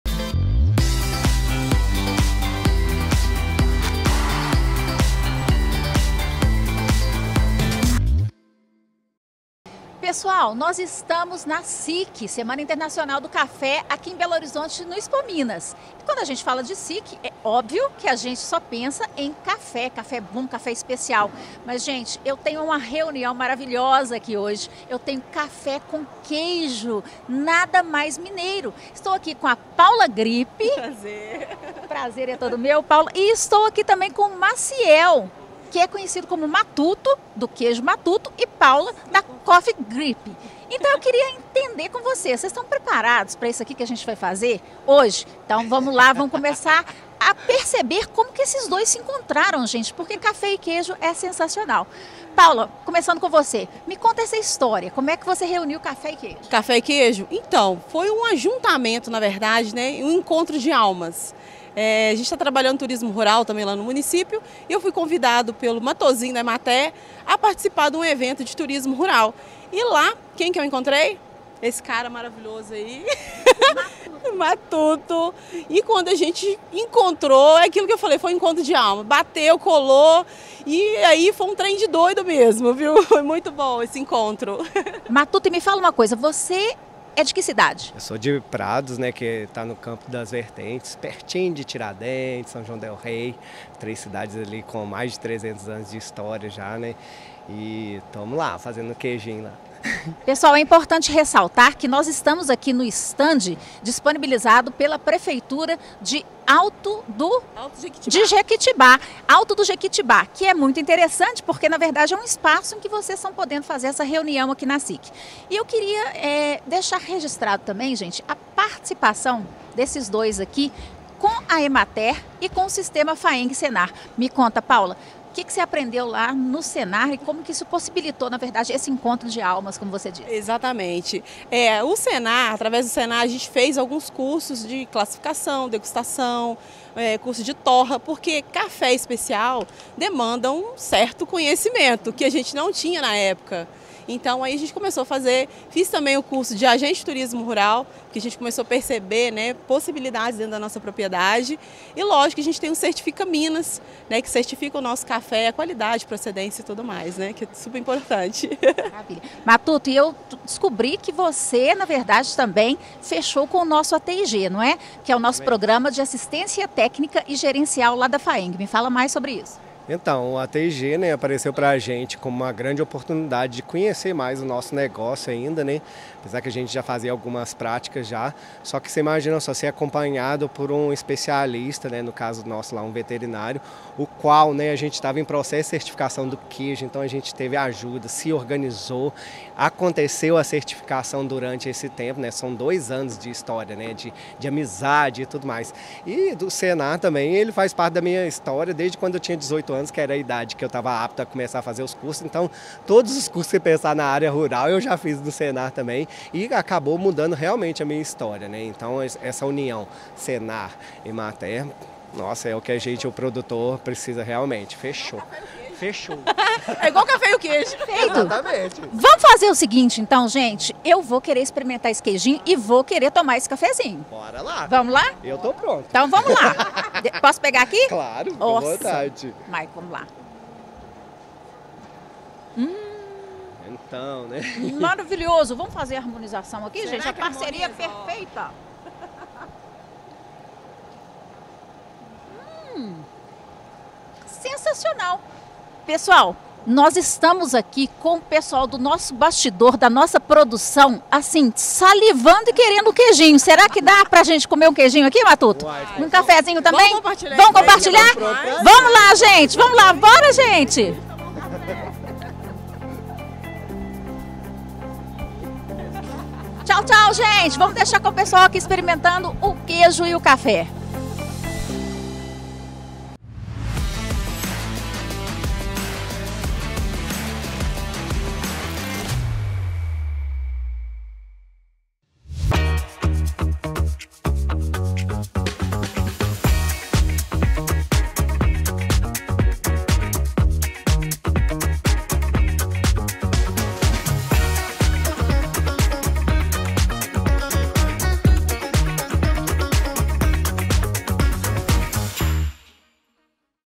SIC 2024 | Harmonização café e queijos
Trabalho desenvolvido pela EMATER e Sistema Faemg Senar garante harmonização perfeita entre café e queijo.